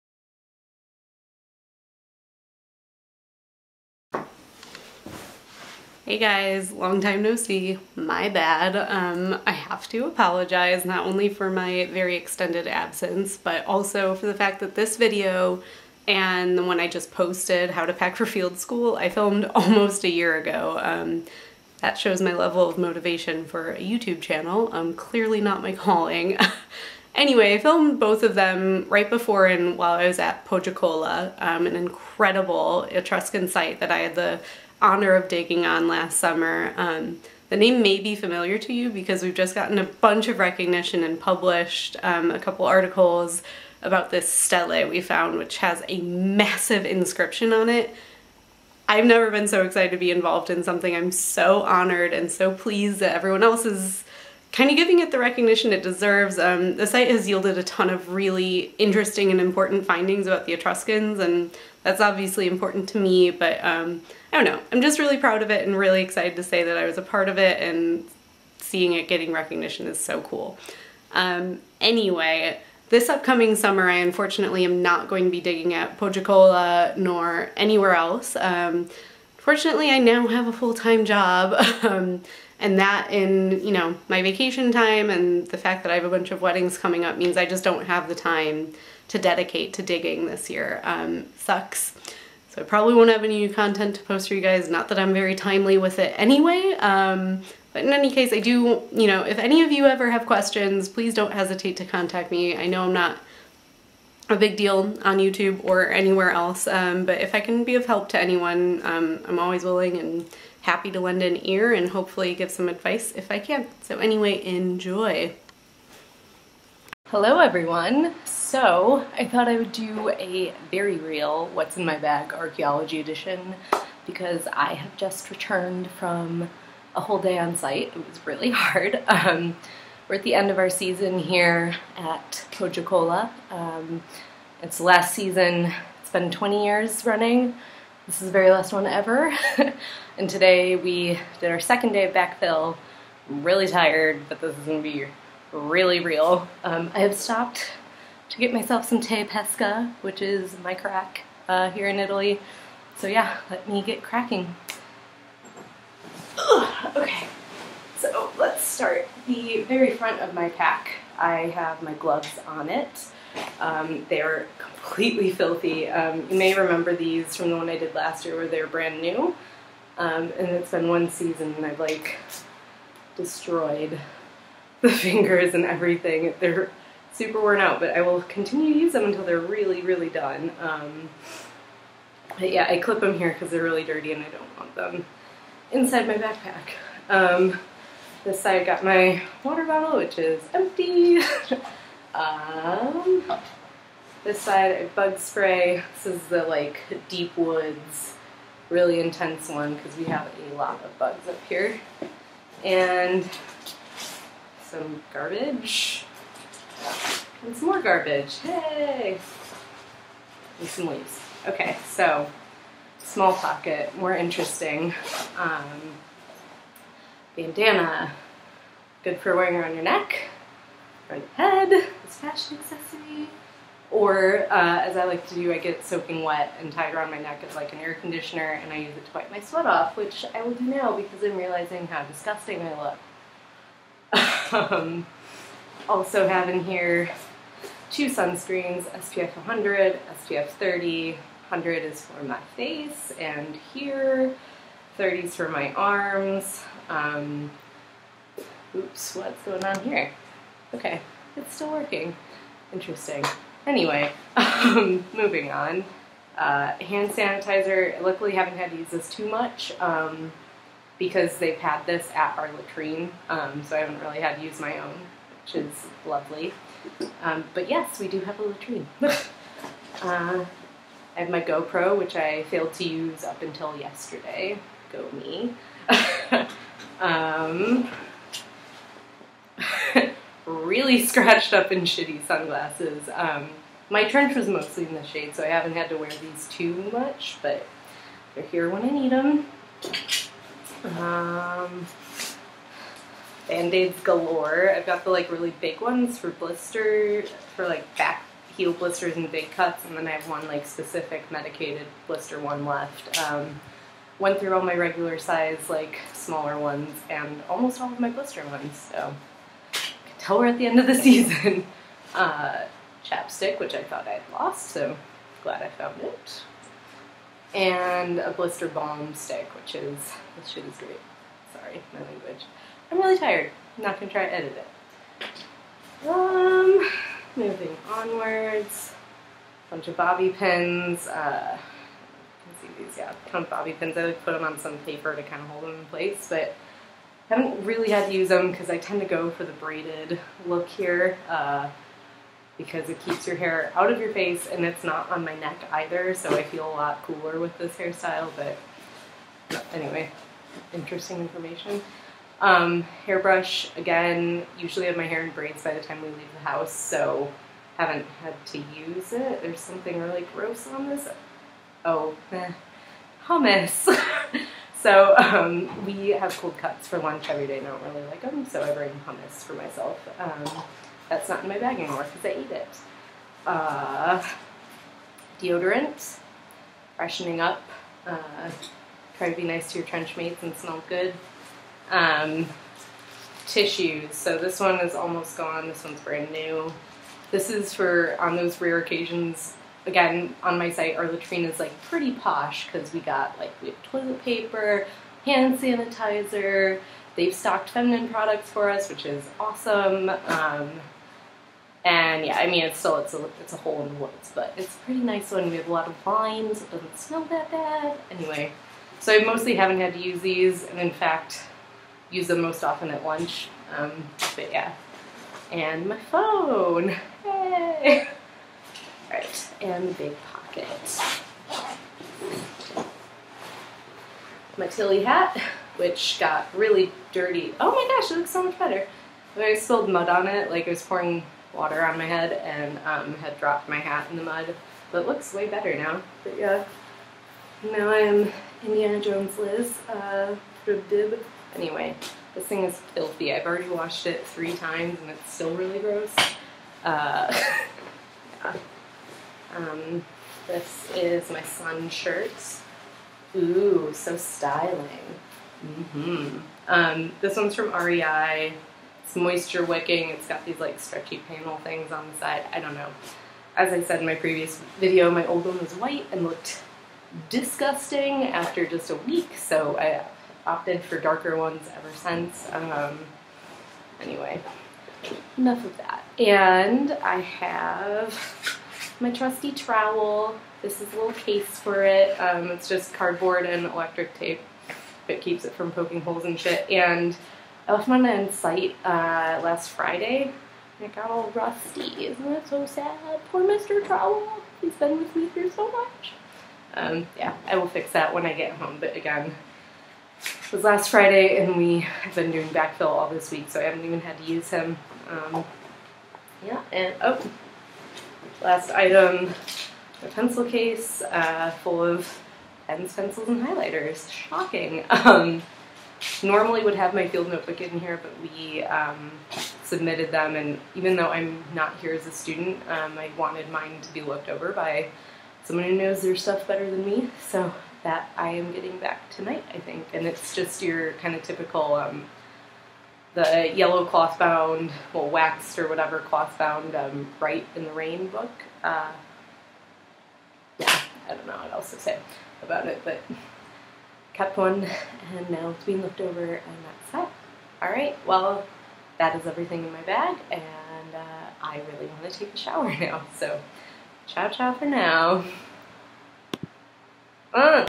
What's in my Bag 2.0: Archaeology Excavation Edition
Super sweaty and covered in dirt, I'm here to show you what's in my bad after a day on site.  (...yep, still no whips or pistols)

→ READ ME ←

-=camera=-
filmed with: Canon Powershot SX130 IS and webcam (shh!)